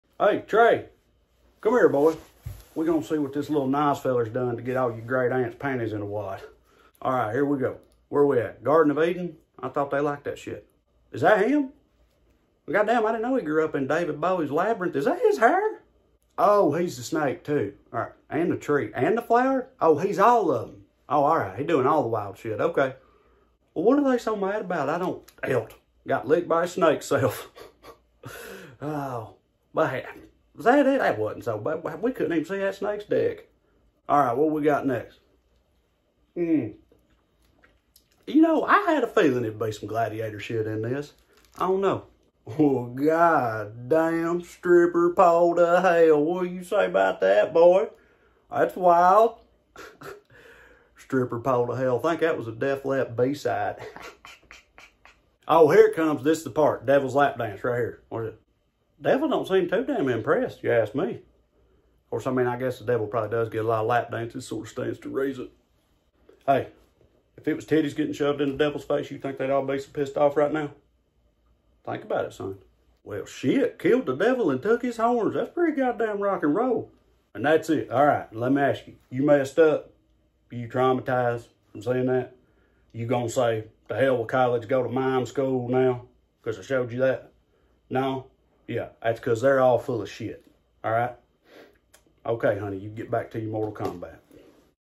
How My Godless Redass Daddy Woulda Handled the Lil Nas X Situation
My dad hated censorship with a passion and showed me R-rated movies and bought me unedited Eminem records and all that rad stuff. Can’t help but laugh at the pearl-clutching and “someone think of the children”ing going on over Lil Nas X right now.